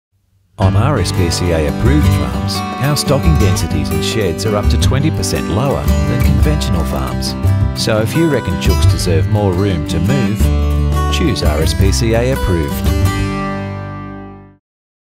For creature comforts, choose RSPCA Approved
To find chicken that's been farmed humanely look for RSPCA Approved.

The RSPCA Approved Farming Scheme Standards ensure that animals in these farming systems are provided with an environment that allows them to do the things that come naturally.